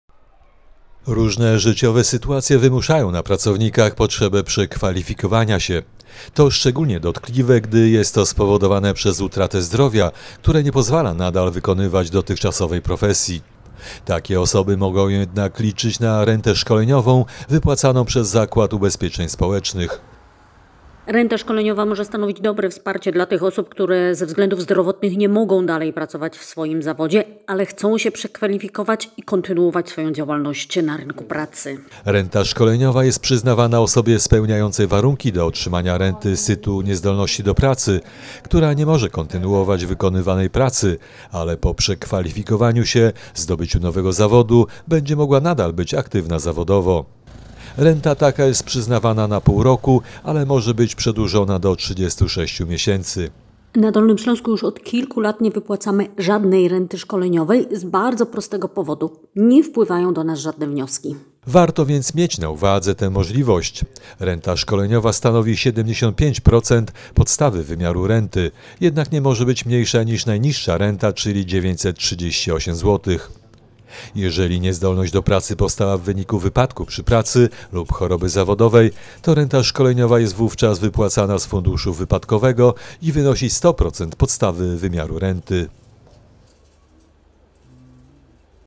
Renta szkoleniowa z ZUS
Renta szkoleniowa to świadczenie Zakładu Ubezpieczeń Społecznych, z którego rzadko korzystają świadczeniobiorcy. Na Dolnym Śląsku nikt z niej nie korzysta. A jest to świadczenie, które mogą pobierać na przekwalifikowanie się osoby spełniające warunki do otrzymania renty z tytułu niezdolności do pracy.